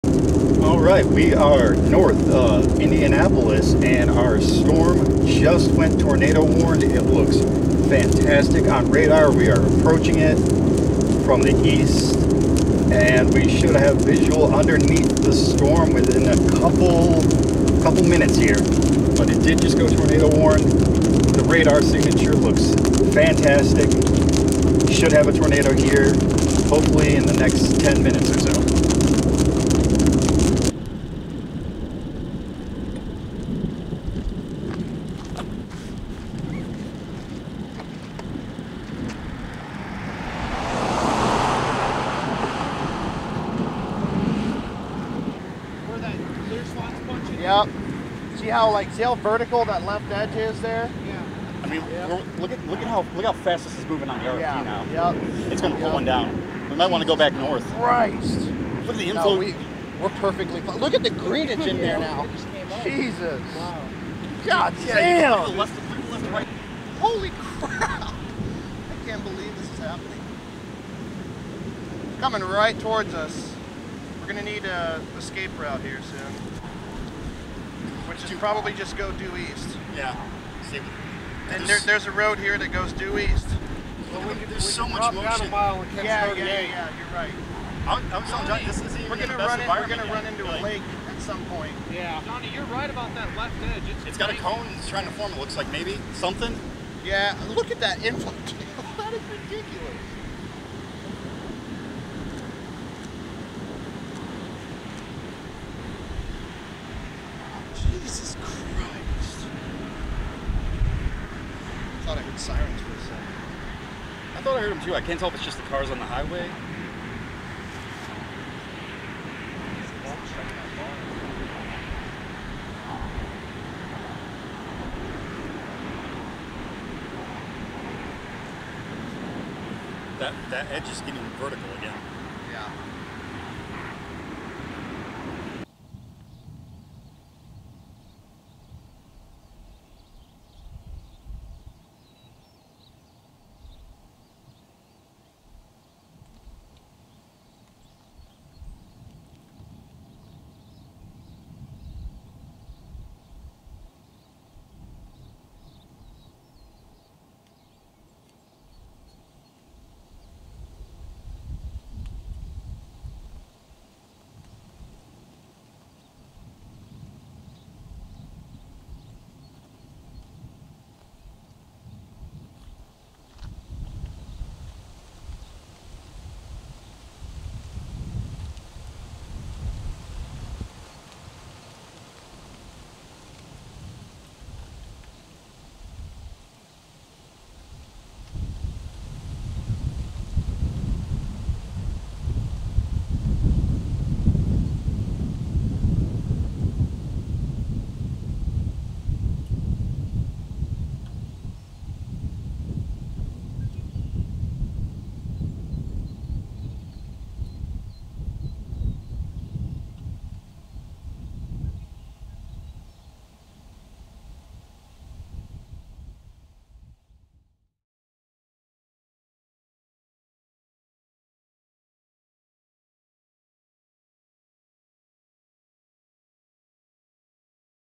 Indiana Supercells - June 25th, 2023
On this day I cased my highest tornado risk of 2023 thus far; a 10% hatched in the Ohio/Indiana border. Although the storm we intercepted north of Indianapolis was tornado warn and look good both visually and on radar, it failed to produce a tornado. On my way back to Ohio I intercepted a small LP (low precipitation) supercell that popped up near Hartford City, Indiana.